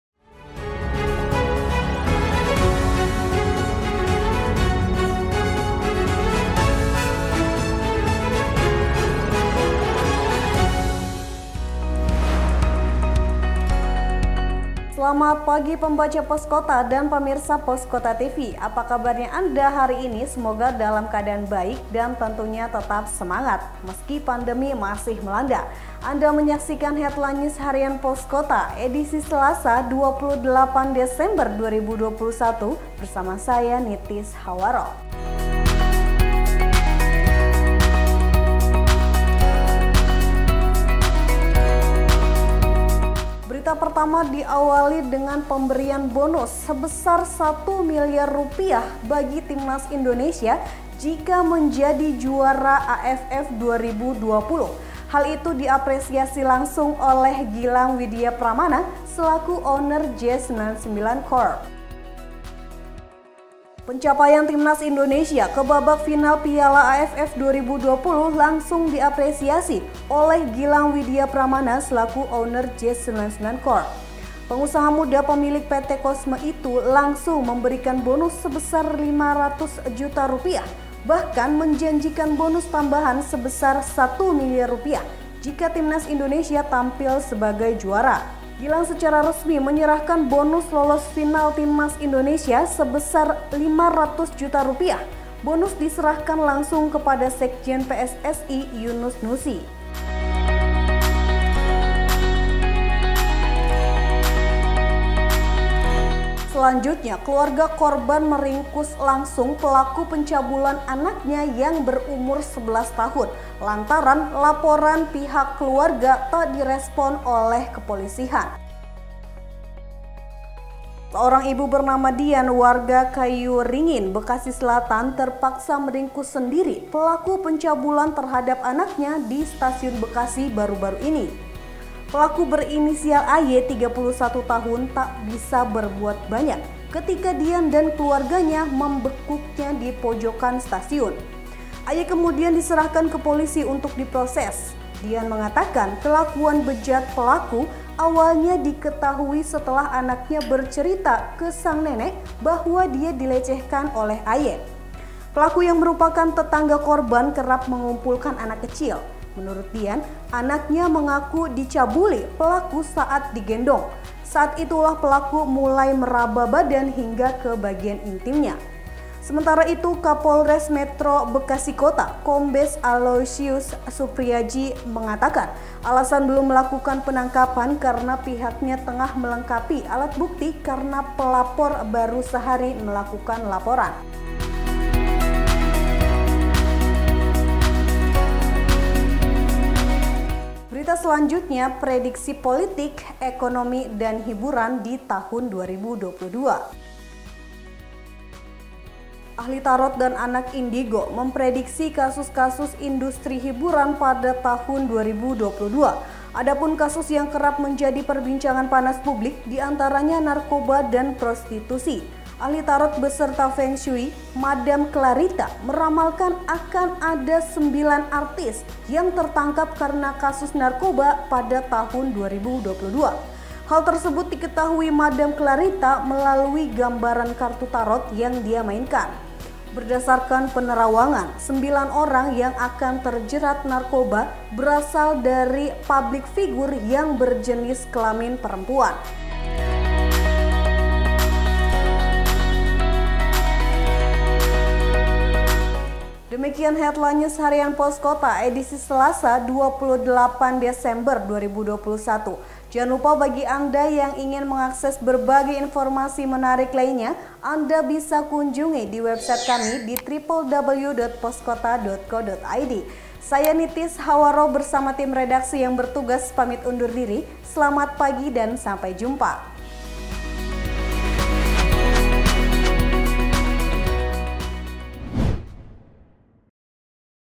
HEADLINE HARIAN POSKOTA EDISI SELASA 28 DESEMBER 2021
Harian Poskota menyajikan beberapa topik menarik di antaranya Timnas Indonesia Dijanjikan bonus Rp 1 miliar oleh J99 Corp, keluarga korban menciduk pelaku pelecehan seksual anak 11 tahun dan prediksi Politik hiburan dan ekonomi di tahun 2022.

Juga berita-berita khas rubrikasi Poskota akhir pekan seperti UMKM, komunitas, kuliner, plesir dan otomotif.